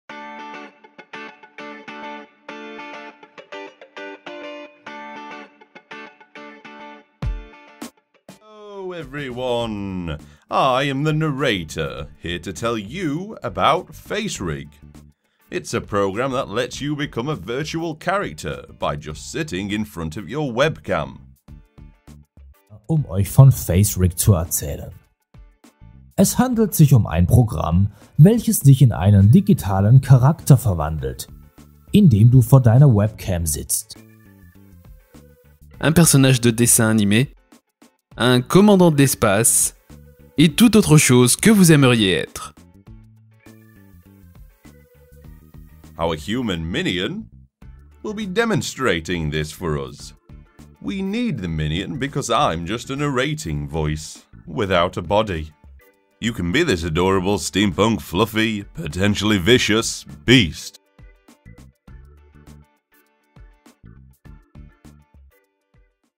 DevBits 11: Talking Fluffo. Sound driven phoneme animations.
Here is Fluffo whose lips are moving based on a mix of expression tracking and real time sound-based phoneme recognition. It is not language-dependent. This is done with a third party signal analysis SDK on top of which we layer our animation system.